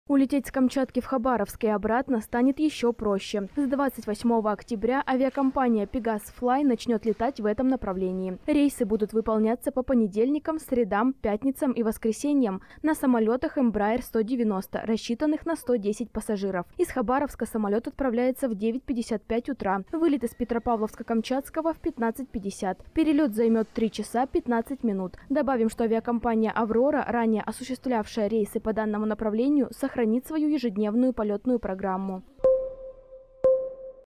Дополнительные рейсы в Хабаровск | Новости сегодня | Происшествия | Масс Медиа
Улететь с Камчатки в Хабаровск и обратно станет еще проще – с 28 октября авиакомпания Pegas Fly начнет летать в этом направлении. Рейсы будут выполняться по понедельникам, средам, пятницам и воскресеньям на самолетах Embraer 190, рассчитанных на 110 пассажиров. Из Хабаровска самолет отправляется в 09:55, вылет из Петропавловска-Камчатского в 15:50. Перелет займет 3 часа 15 мин. Добавим, что авиакомпания «Аврора», ранее осуществлявшая рейсы по данному направлению, сохранит свою ежедневную полётную программу.

Есть о чем рассказать? Пиши нам в Telegram – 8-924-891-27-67 или в Direct